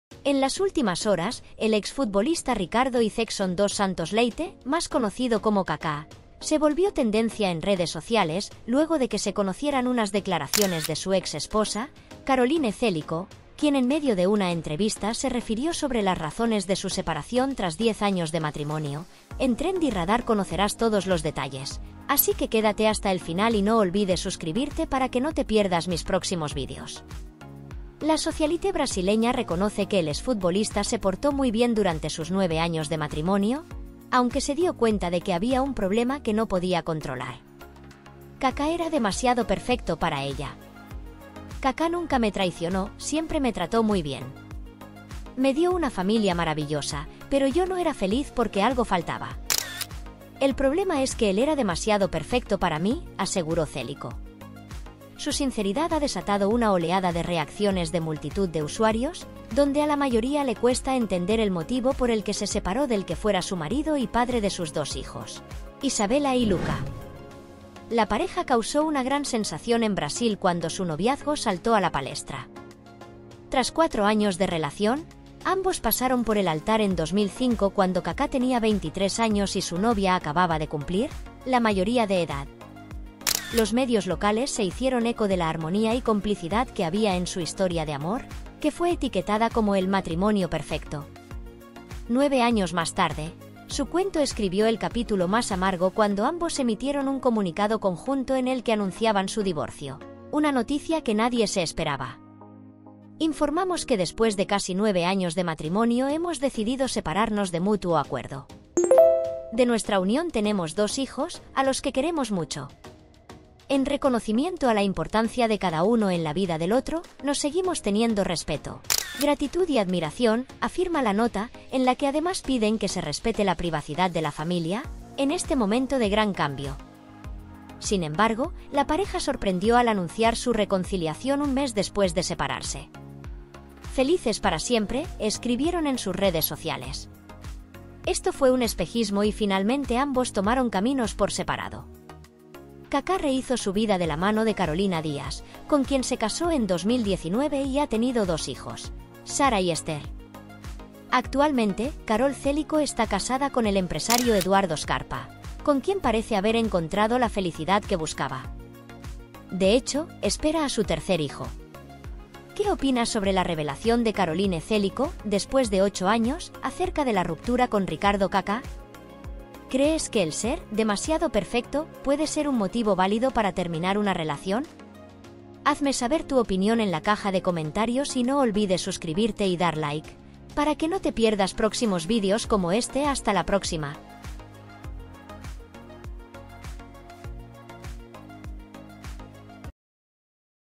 😱 Exmujer de Kaká Revela Secreto del Divorcio
Caroline Celico se ha sincerado y ha explicado la razón que le llevó a divorciarse de Kaká en 2014.

👉 Otros Videos que te gustarán en el mundo del espectáculo